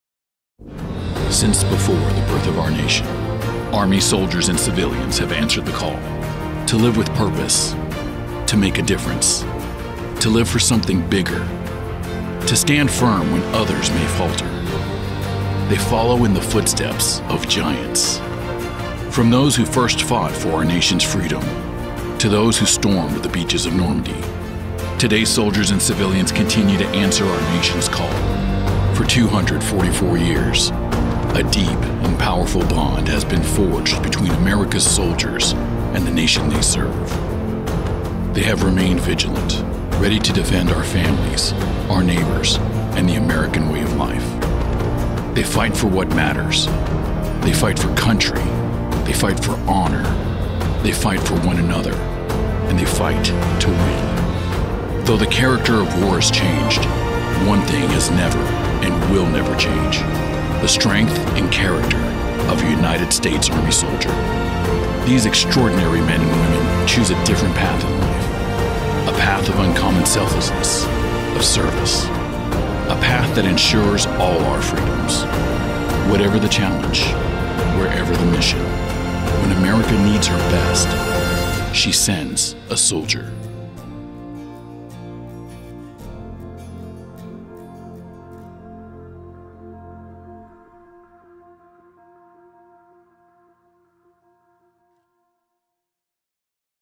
Army Birthday: 244 years of defending and protecting America
On June 14, 2019, we celebrate 244 years of defending and protecting America, and salute the generations of Soldiers who have answered the call to serve. Learn about the America's First National Institution